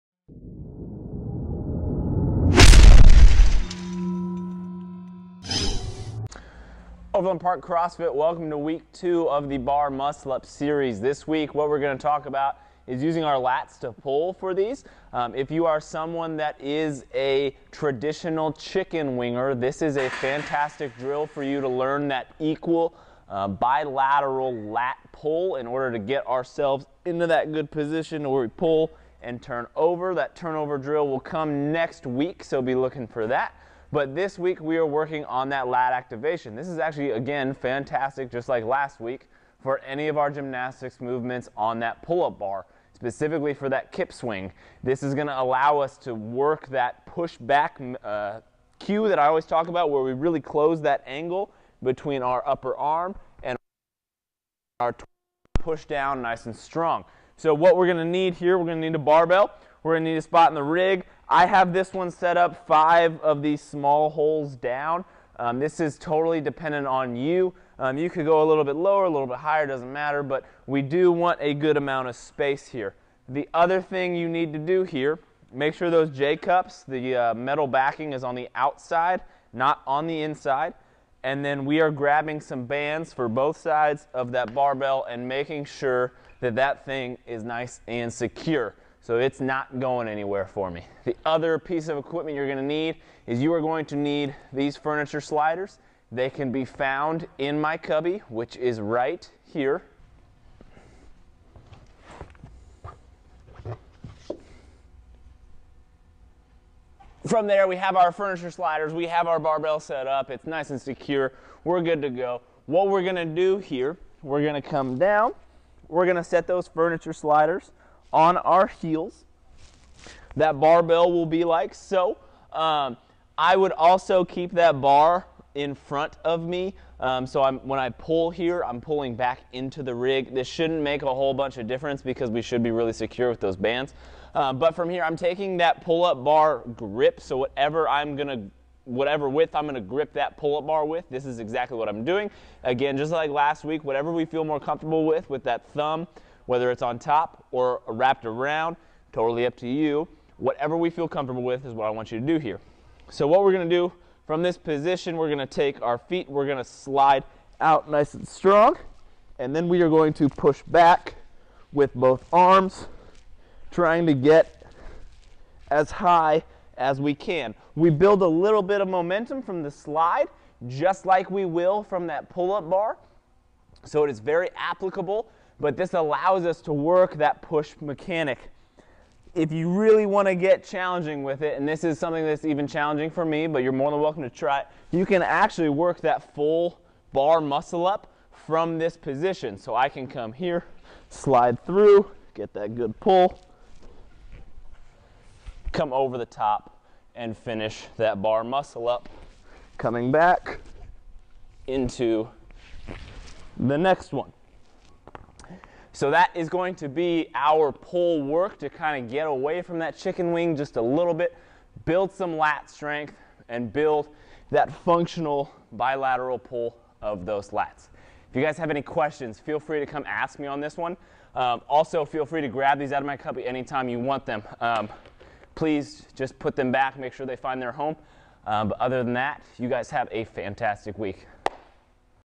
Overland Park CrossFit - Bar muscle up (Part 2 - Lats)
This video is part 2 of a series, on performance pointers about bar muscle ups and exercises that will increase your strength and ability to perform this movement correctly.  This week's video will address improving strength to be able to achieve an equal bilateral lat pull during the movement.